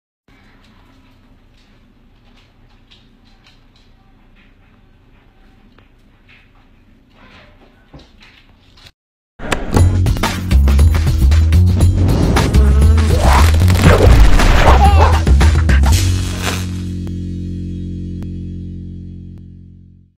Clever dog leans how to open motion sensored trash cam
It's clear that Pearl has figured out how to open the motion sensor trash. Check it out!